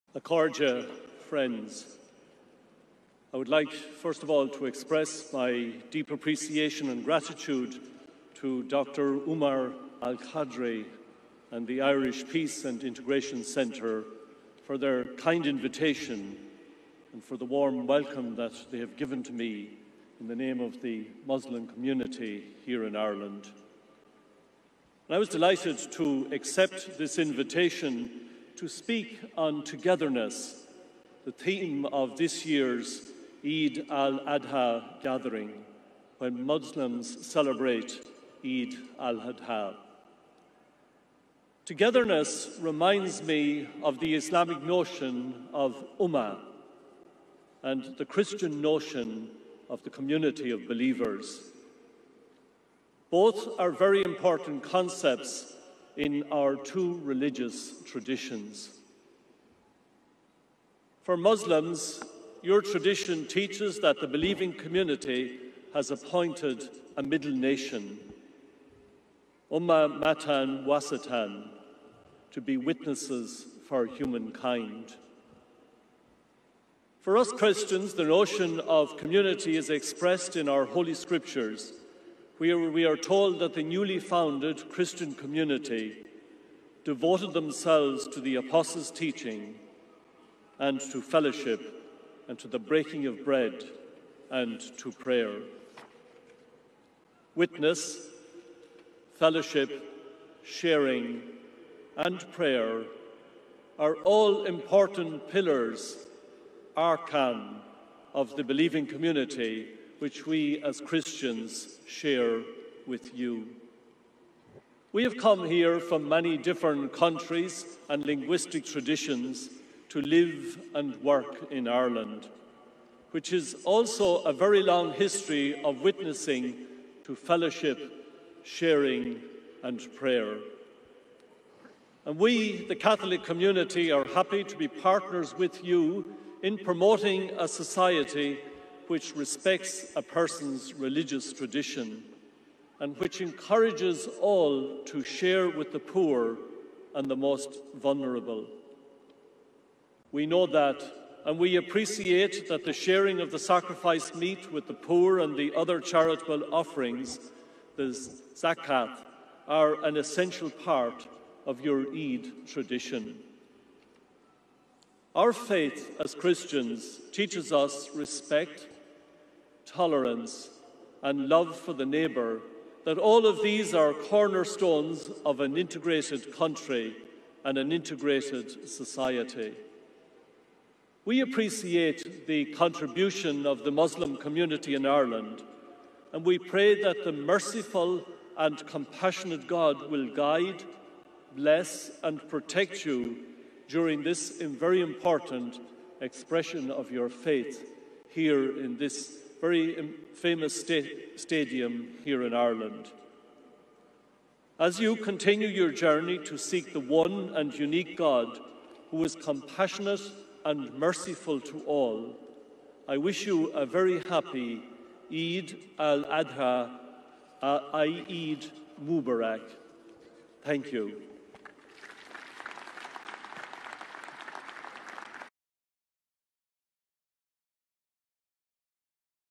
Archbishop Dermot Farrell addresses Eid at Croke Park 2021
The Catholic Archbishop of Dublin, Archbishop Dermot Farrell addresses 500 Irish Muslims at the Eid in Croke Park 2021 celebrations in Ireland's historic and iconic Croke Park Stadium in Dublin.